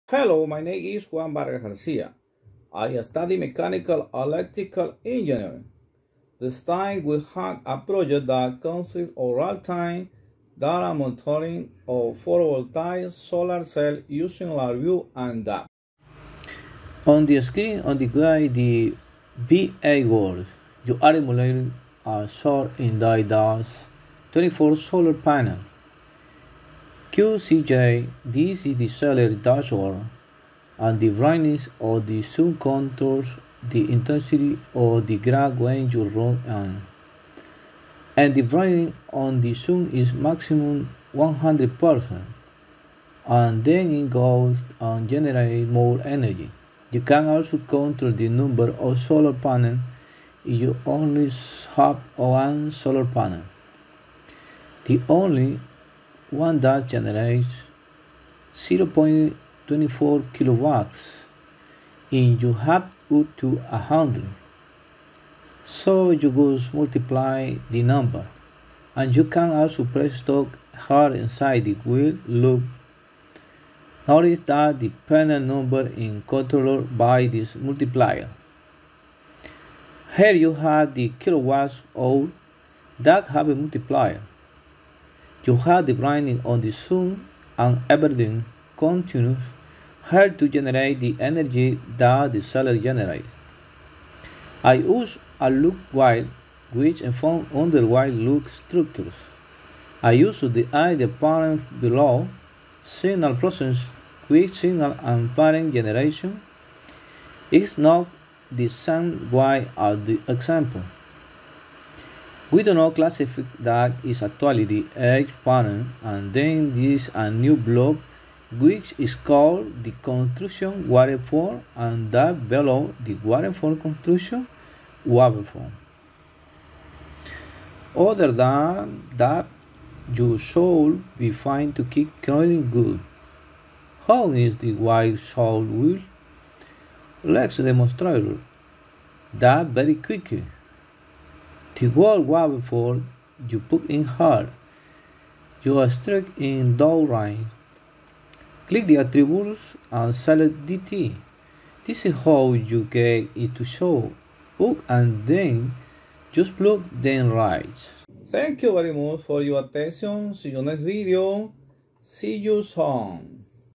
Real Time Data Monitoring of PV Solar Cell Using LabView and DAQ
Hello. a new video about Real Time Data Monitoring of PV Solar Cell Using LabVIEW and DAQ.